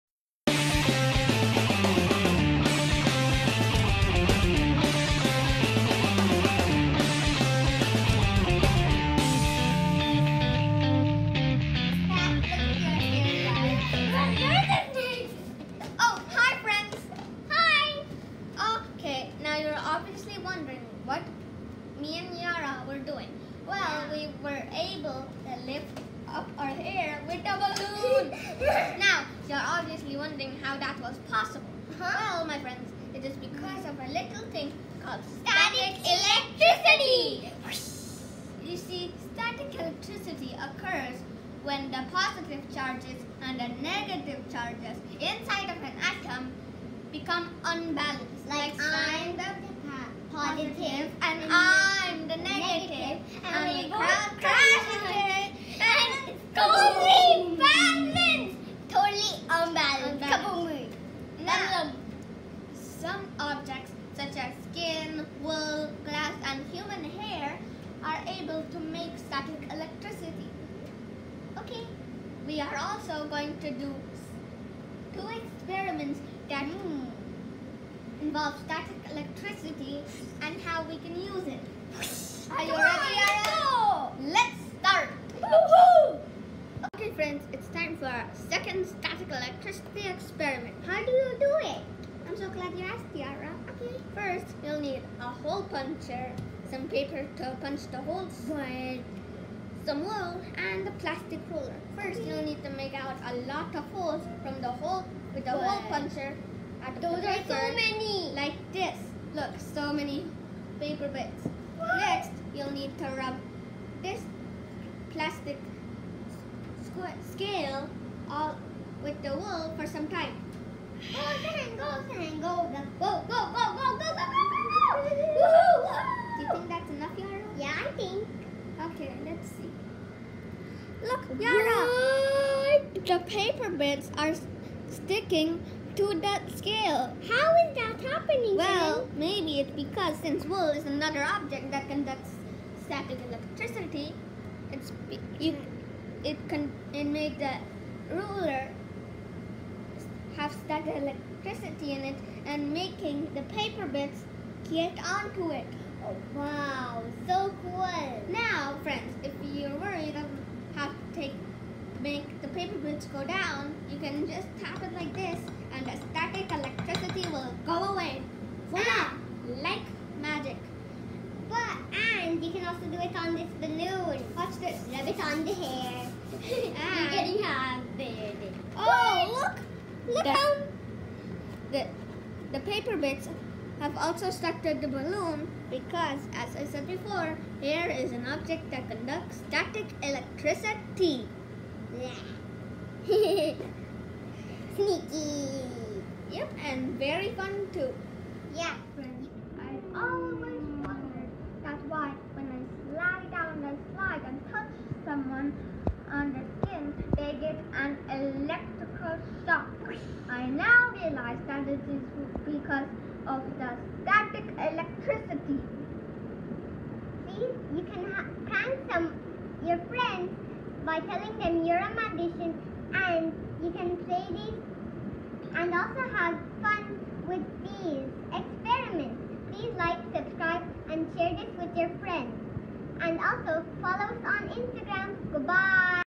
Static experiments for kids to try at home.|Static hairdos |Magical ruler
Are you ready for some magical experience with science?join us for some fun activity with static electricity using simple materials like balloons,plastic ruler,wool, paper hole punching pad ,paper .